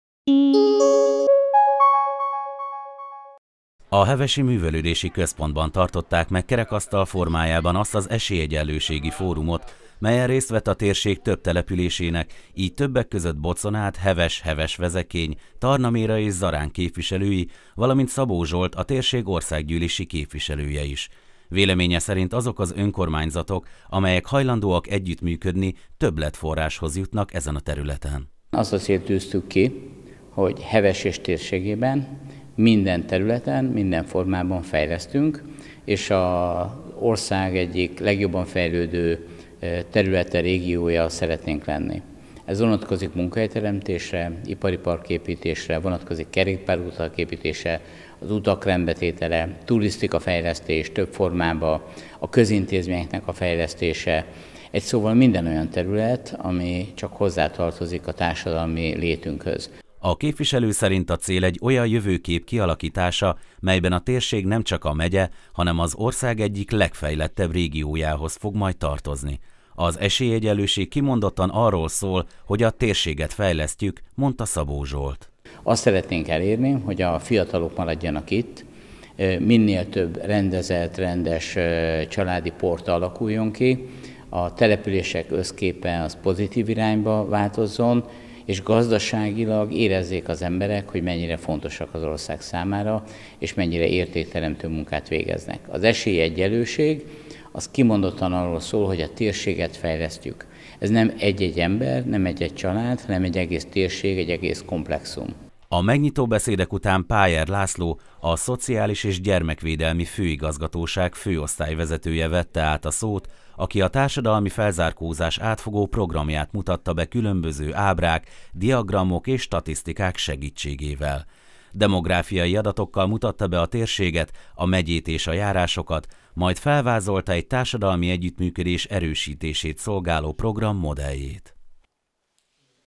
Esélyegyenlőségi fórumot tartottak Hevesen
Esélyegyenlőségi fórumot tartottak Hevesen, ahol a térség önkormányzatainak képviselői tartottak kerekasztal-beszélgetést, ahol célokról és tervekről is szó esett. A fórum jó alkalom volt az együtt gondolkodásra, hogyan lehet kifejleszteni a jó módszereket és technikákat.